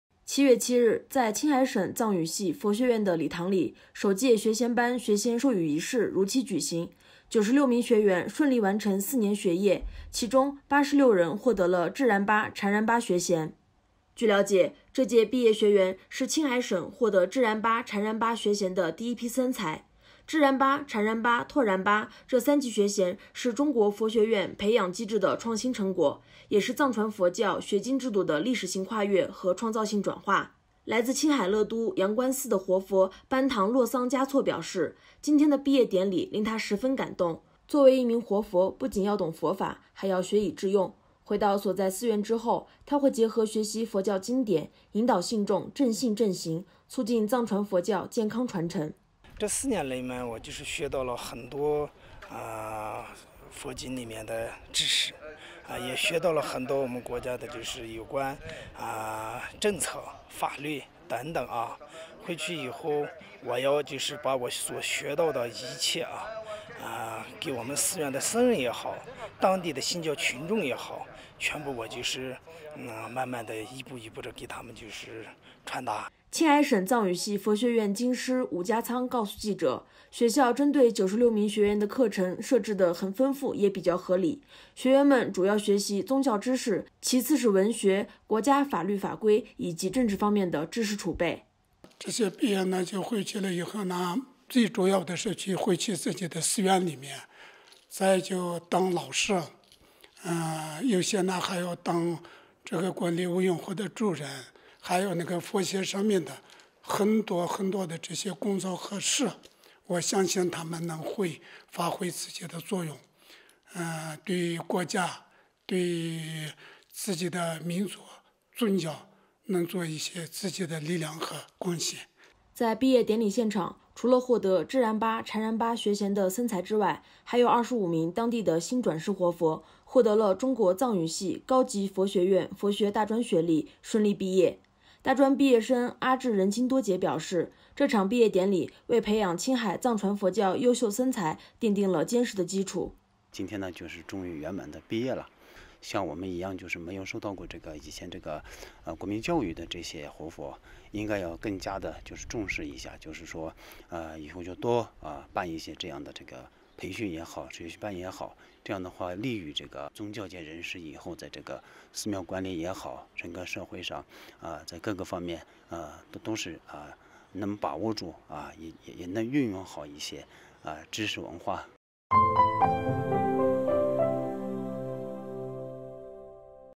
青海省藏语系佛学院首届学衔班学员毕业
7月7日，在青海省藏语系佛学院的礼堂里，首届学衔班学衔授予仪式如期举行。96名学员顺利完成4年学业，其中86人获得了“智然巴”、“禅然巴”学衔。